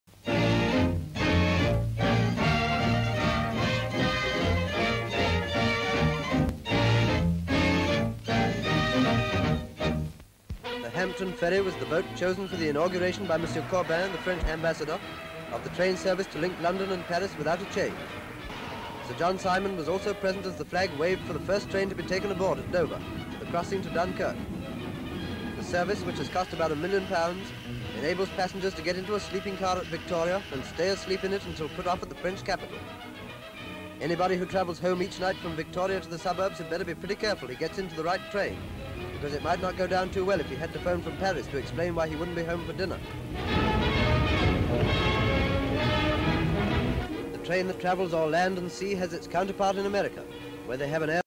New train ferry service from London to Paris (1936)
Sir John Simon (Lord High Chancellor) attends the 1936 inauguration of the Hampton Ferry, the new train ferry from London to Paris

Full Description:

SLATE INFORMATION: The Train Ferry - New direct service from London to Paris

ENGLAND: Kent: Dover:
EXT

DOVER Opening of Channel Ferry Rail service by French Ambassador

RAILWAYS Coaches for the Channel Rail Ferry & opening at Dover by French Ambassador

SIMON, Sir John At opening of Channel train ferry Service

ferries, trains, rail, transport, transportation, 1st Viscount Simon, ceremony, ceremonies
Background: Sir John Simon (Lord High Chancellor) attends the 1936 inauguration of the Hampton Ferry, the new train ferry from London to Paris